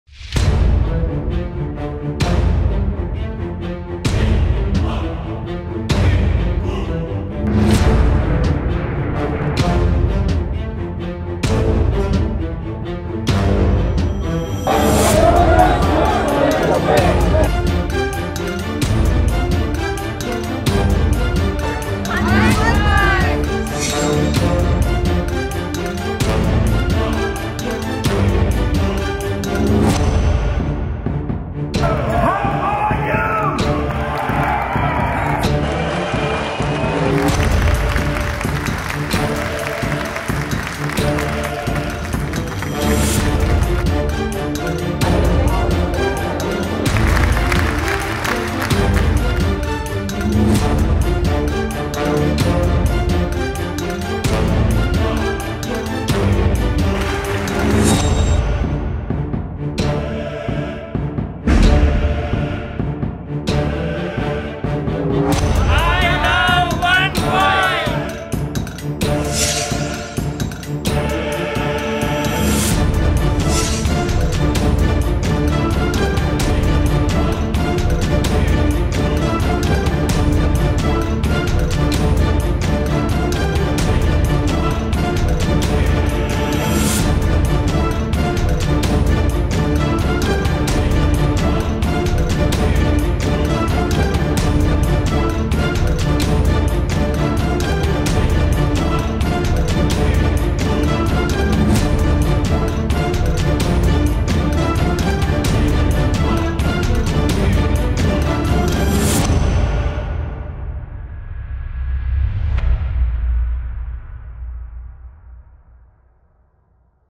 Amazing OneCoin Event in DUBAI
OneCoin.eu event in Dubai with over 3000 guests